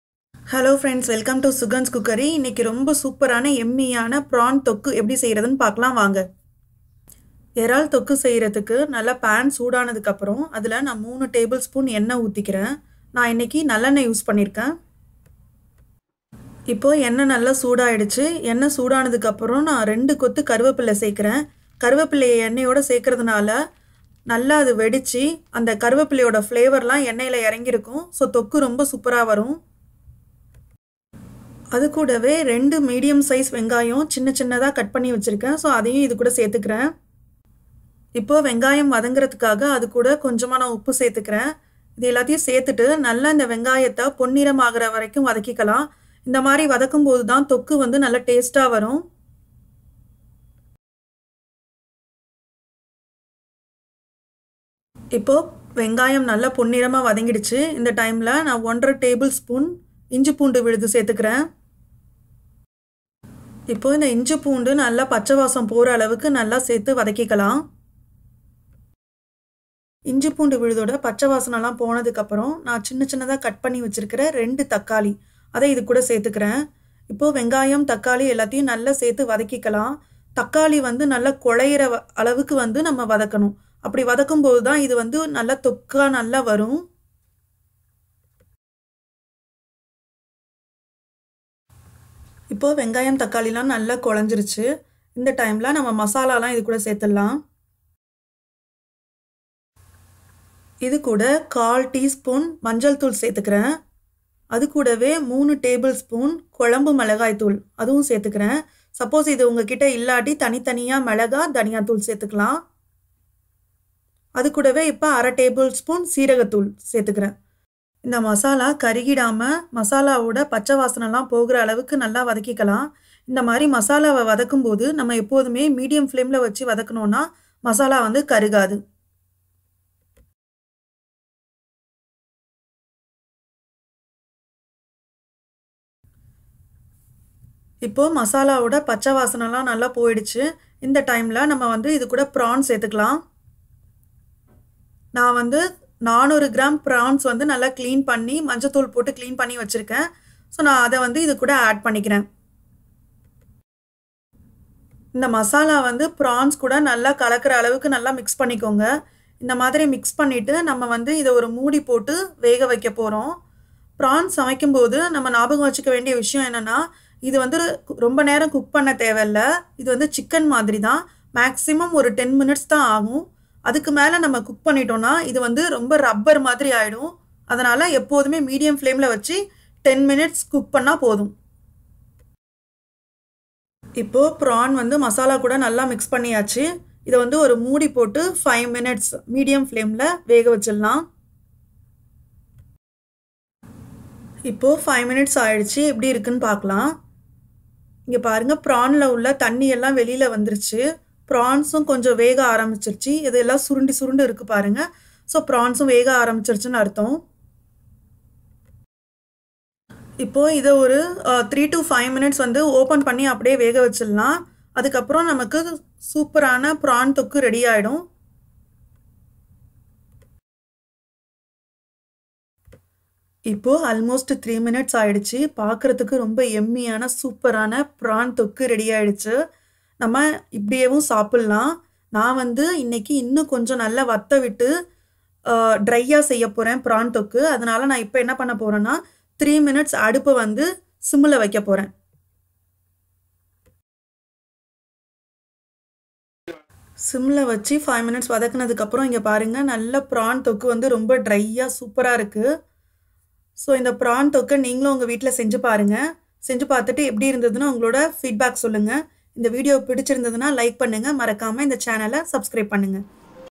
Prawn Thokku in Tamil | இறால் தொக்கு | Prawn Varuval in Tamil | Eral Thokku in Tamil
In this Video, We can see how to prepare Prawn thokku/ Prawn Fry dry like restaurant style.

It is very yummy and simple recipe.